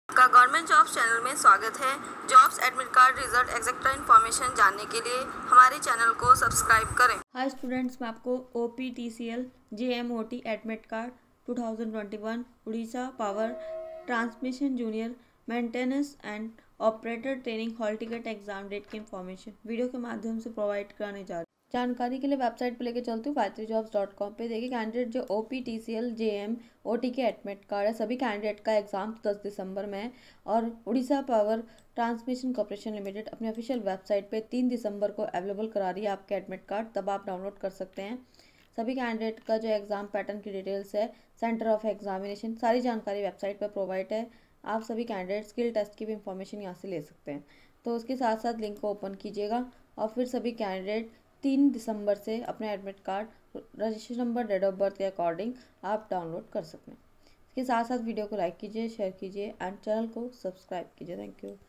OPTCL JMOT Admit Card 2021 Odisha Power Transmission Jr Maintenance & Operator Trainee Hall Ticket
The Odisha Power Transmission Corporation Limited has been uploaded the OPTCL JMOT Admit Card 2021 for the written examination.
OPTCL Advt. No 05/2021 Exam Date -10 December 2021
Download OPTCL JMOT Admit Card 2021: Available On 03 December 2021
ADVT. NO: MPP&R- 05/2021
The aspirants who had applied for 200 posts of Junior Maintenance & Operator Trainee by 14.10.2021 may soon download the OPTCL JMOT Written Exam Admit Card directly through the official link available